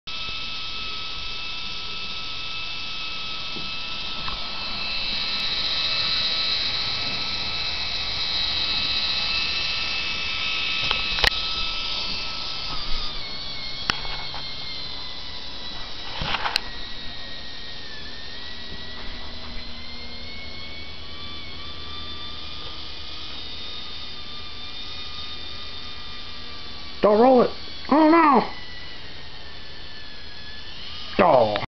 knex 1976 chevy scottsdale
full 4x4 1/12th scale model of a lifted 1976 chevy scottsdale truck. gearing is super low. great for rockcrawling.